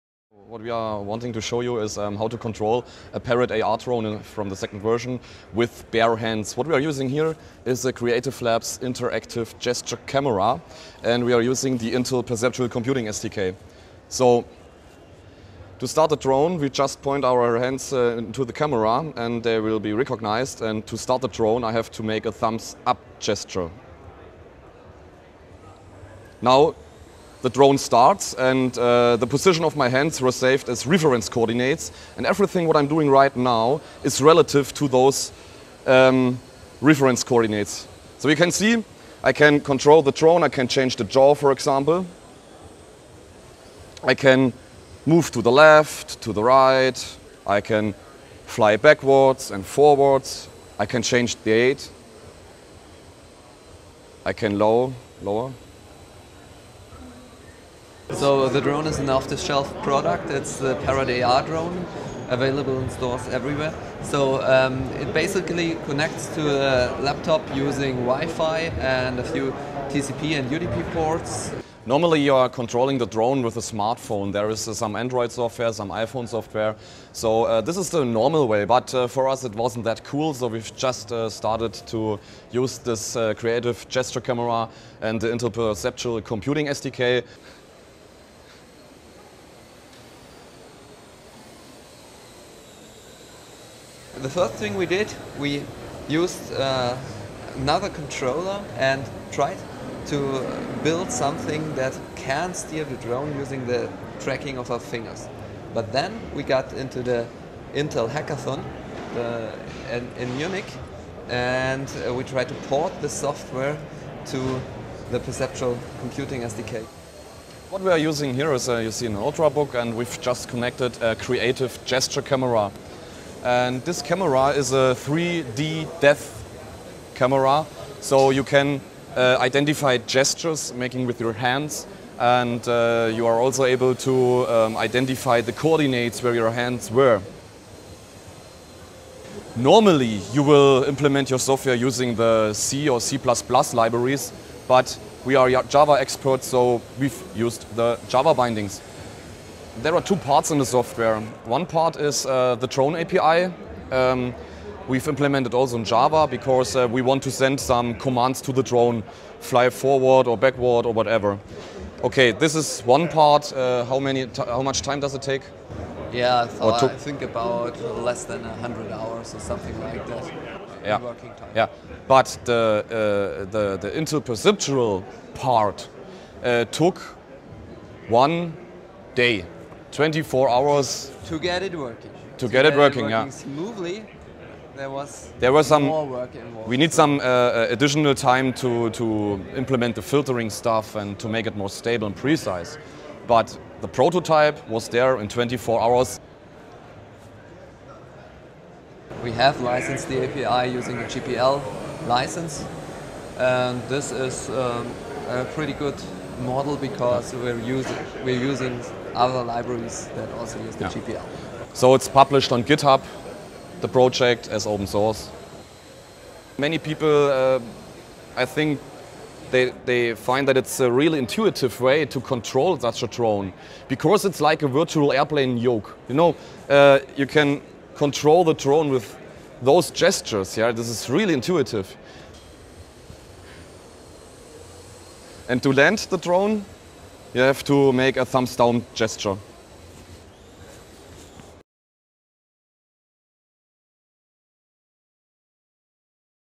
Gesture Controls Pilot Drone
Martin Förtsch and Thomas Endres developed an application with Java code and the Intel Perceptual Computing SDK that uses a 3-D camera and a Wi-Fi connection to pilot a Parrot AR drone with hand motions.

Drones Go 'Hands Free' with Gestural Technology
Software developers use 3-D camera, open source application to fly drone with hand motions.

Drones are buzzing in the sky above everywhere from Afghanistan to amateur sporting events. Typically, the unmanned aerial vehicles are controlled with a computer keyboard, game controller or smartphone, but a pair of software developers have discarded these familiar interfaces to create a "hands-free" flying experience.

"People find that it is a really intuitive way to control the drone because it is like a virtual airplane yoke," said software developer Martin Förtsch.